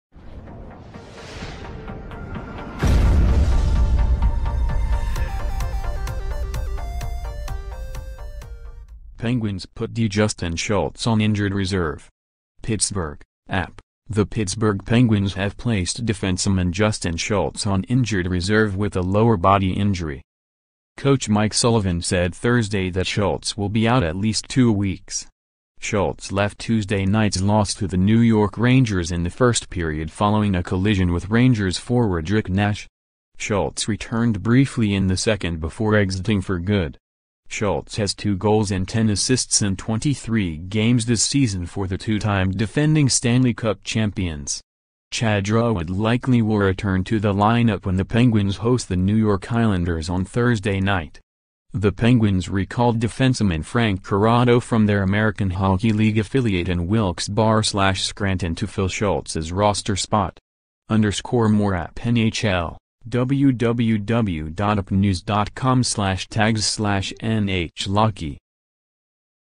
Penguins put D Justin Schultz on injured reserve
PITTSBURGH (AP) — The Pittsburgh Penguins have placed defenseman Justin Schultz on injured reserve with a lower-body injury.

Coach Mike Sullivan said Thursday that Schultz will be out at least two weeks. Schultz left Tuesday night's loss to the New York Rangers in the first period following a collision with Rangers forward Rick Nash. Schultz returned briefly in the second before exiting for good.

Schultz has two goals and 10 assists in 23 games this season for the two-time defending Stanley Cup champions. Chad Ruhwedel likely will return to the lineup when the Penguins host the New York Islanders on Thursday night.